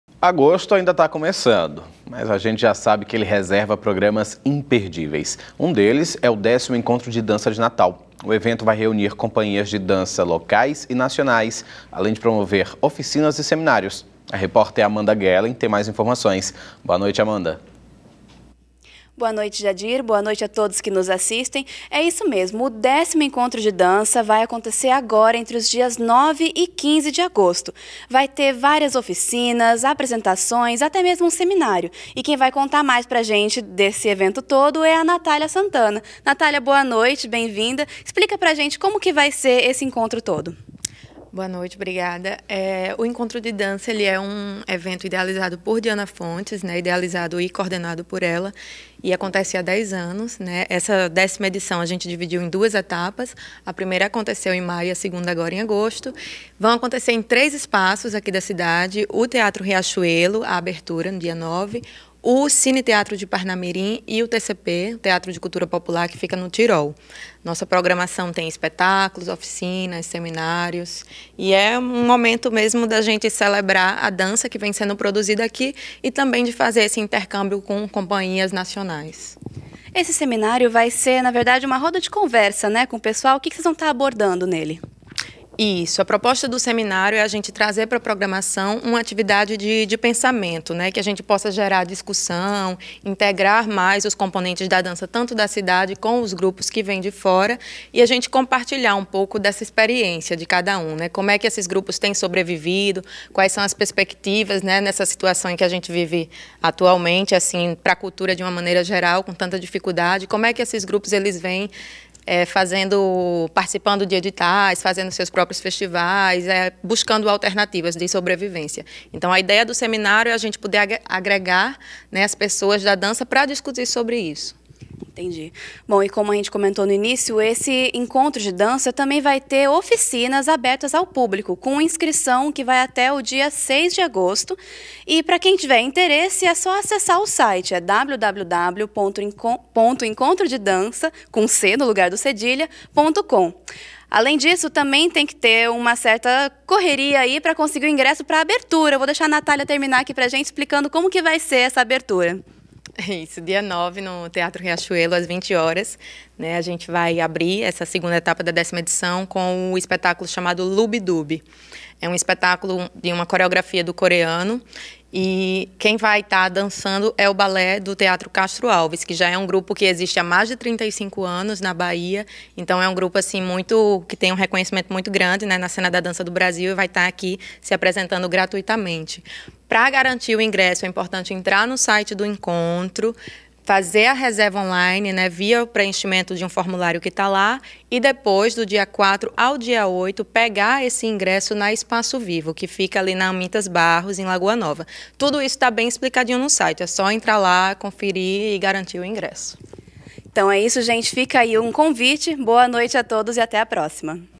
Encontro de Dança
Programa exibido 02/08/2017.

Encontro de dança reunirá companhias locais e nacionais em Natal para evento gratuito.

Repórter: Amanda Gehlen